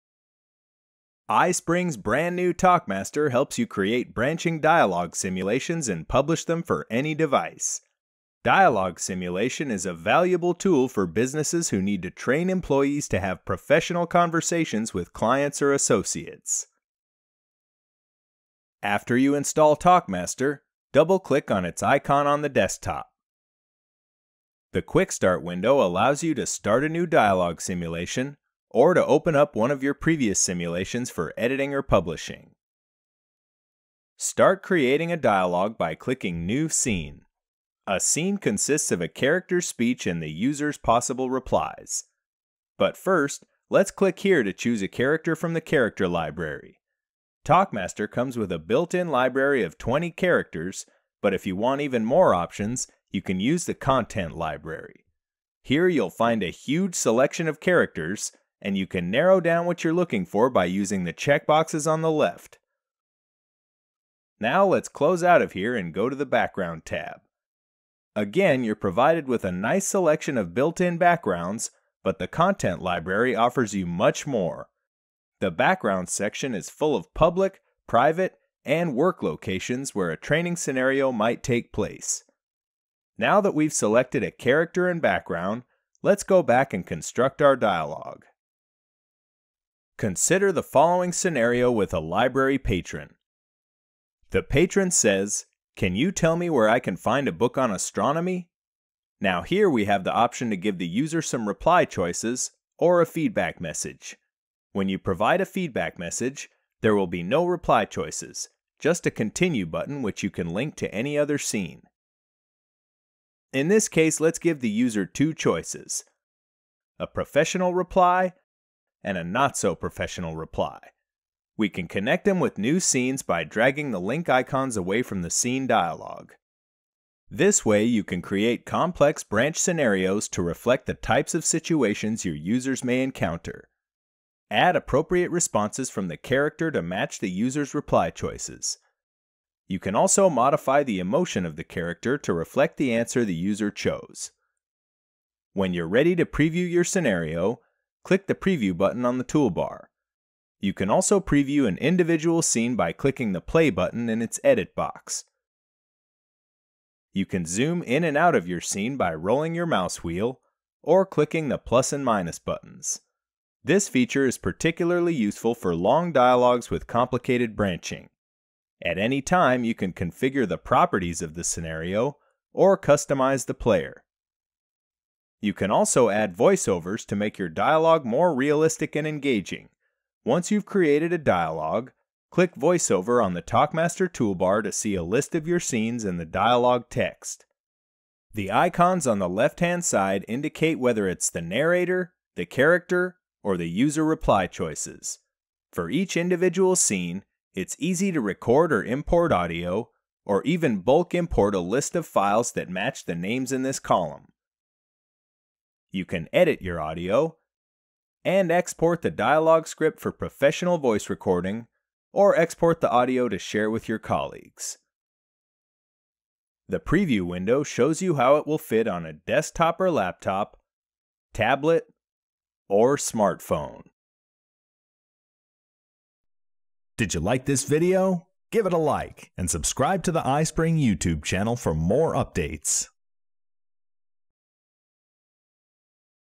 iSpring TalkMaster Overview (Standalone)
Dialogue simulation is a valuable tool for businesses who need to train employees to have professional conversations with clients or associates.

This video shows you how to get started creating dialogue scenarios right away and publish them for any device.